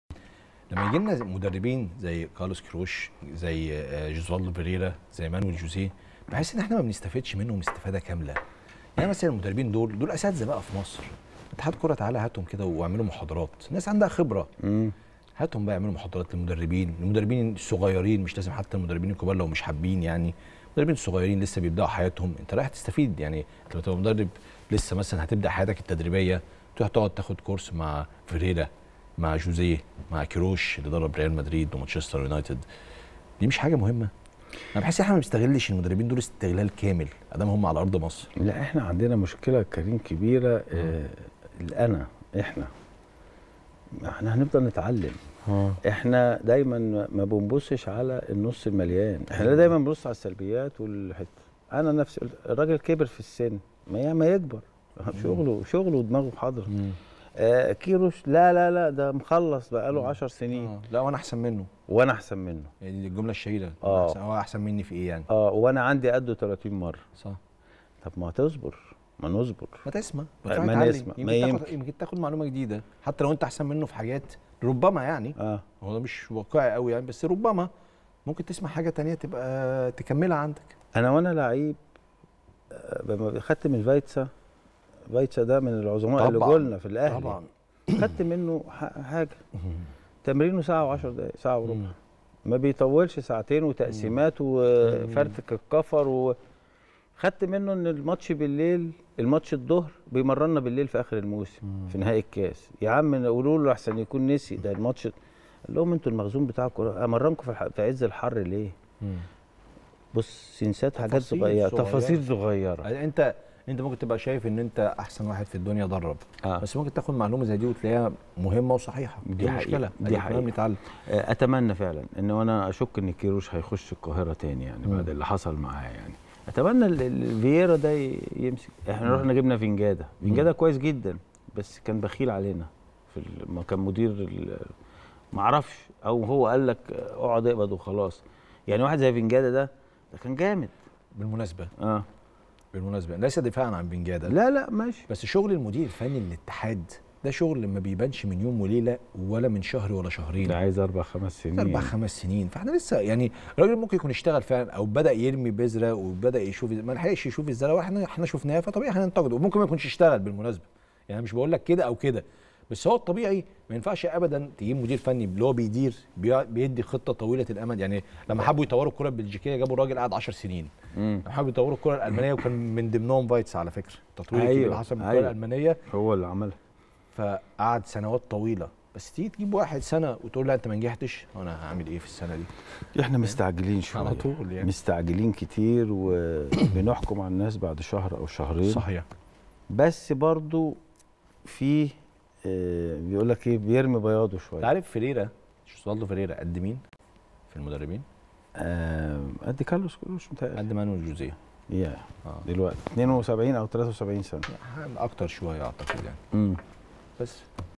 كريم رمزي: أتمنى المدربين الصاعدين الاستفادة من “فيريرا - مانويل جوزية- كيروش” وعبد الجليل يعلق
رقم 10 | كريم رمزي: أتمنى المدربين الصاعدين الاستفادة من “فيريرا - مانويل جوزية- كيروش” وعبد الجليل يعلق